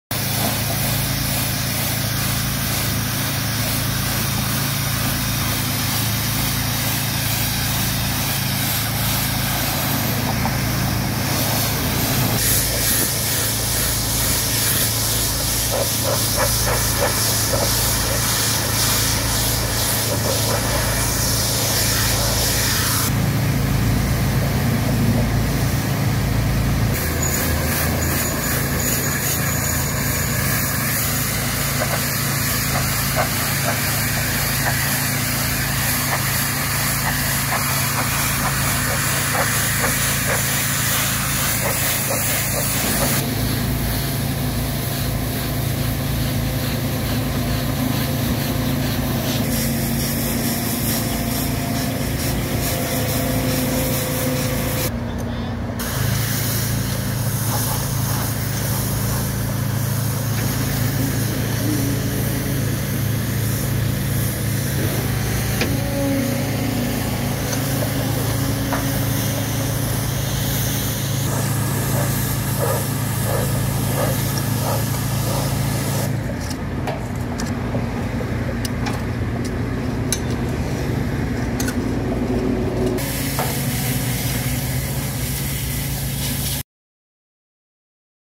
NICHOLS SAWMILL BRIDGE WASHING 111221 final
Commissioner Charlie Riley’s Precinct 2 team cleaned another bridge in the community. The cleaning began after the morning rush hour and start of school. This project included smoothing and clearing away the easement behind the guardrails and bridge railing. The buildup between the posts is smoothed out to allow water to flow off of the road. Precinct 2 strives to provide safe driving conditions for all of the community.